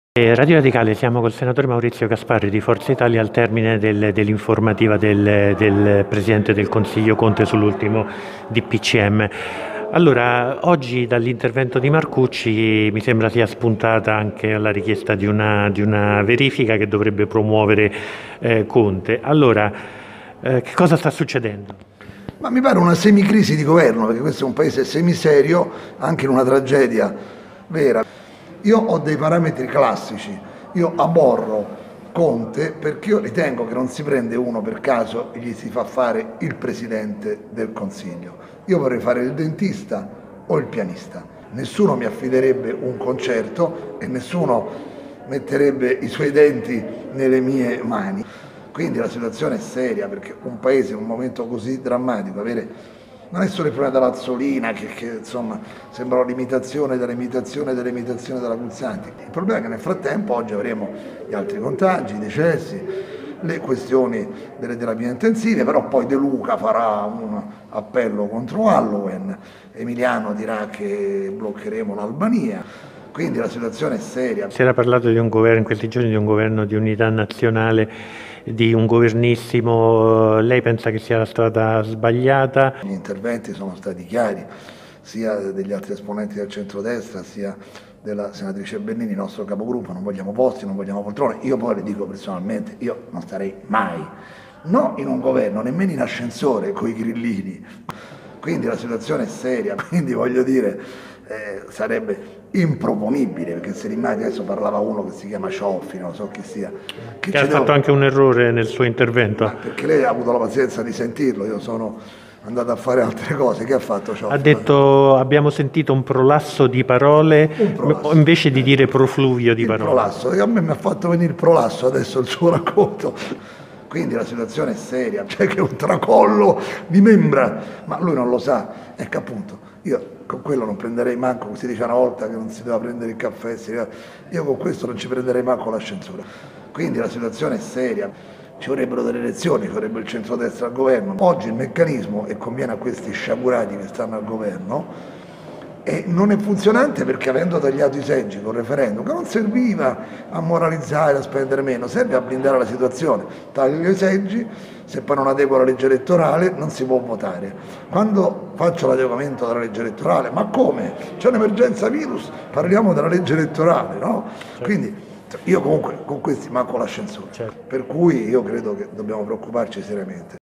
Covid, Un "Grande" Maurizio Gasparri: siamo un paese semiserio.
Questa pantomima di Gasparri serve un pò a sciogliere e disinnescare la tensione sulla tragedia che stiamo vivendo. In un quadro drammatico della situazione, il senatore in quota FI, si esprime cercando per quanto possibile, di essere serio, ma non ce la fa. Ne esce fuori una comica (semi)seria.